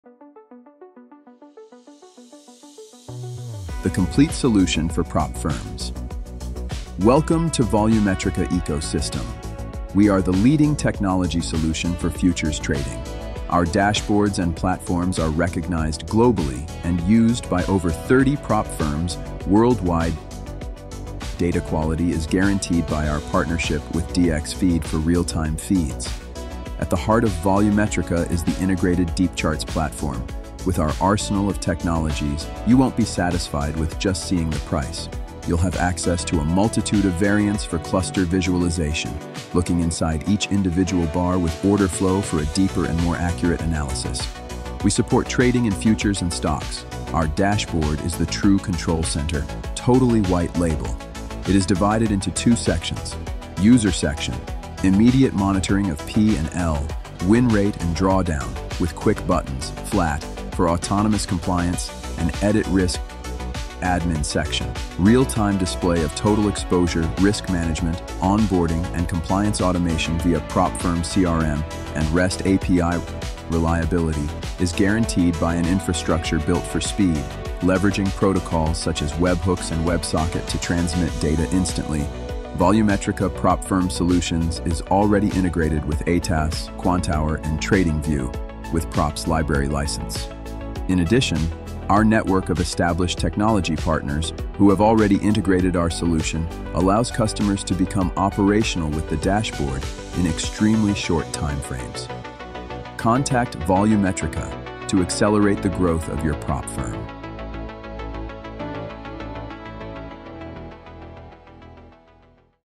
Volumetrica Ecosystem: The Ultimate Tech Solution for Prop Firms
Welcome to Volumetrica Ecosystem – the leading technology solution designed specifically for Futures and Stocks Proprietary Trading Firms. 🌍
Trusted by over 30 Prop Firms worldwide, our ecosystem provides the infrastructure, data, and analytical tools you need to manage risk effectively and offer your traders a superior experience. From our high-end DeepChart platform to our comprehensive White Label Dashboard, we handle the technology so you can focus on growth.
🚀 Key Features of the Volumetrica Ecosystem:
• Integrated DeepChart Platform: Go beyond simple price action. Give your traders access to professional Cluster Visualization, Order Flow analysis, and inside-bar logic for deeper market insights.
• The Ultimate Control Center: A fully White Label Dashboard divided into two powerful sections:
o User Section: Real-time P&L monitoring, Win Rate stats, and instant risk management tools (Flat button, Edit Risk).
o Admin Section: Complete oversight with total exposure tracking, onboarding automation, and automated compliance via PropFirm CRM and Rest API.
• Reliability & Speed: Infrastructure built for high-frequency performance, leveraging Webhooks and WebSocket protocols to transmit data instantly.
• Premium Data Quality: Real-time feeds guaranteed by our partnership with dxFeed.
• Seamless Integrations: Fully compatible with our Desktop and Web Platform, and TradingView (via Props library license), ATAS, and Quantower.
Join the network of established technology partners and get your Prop Firm operational in record time.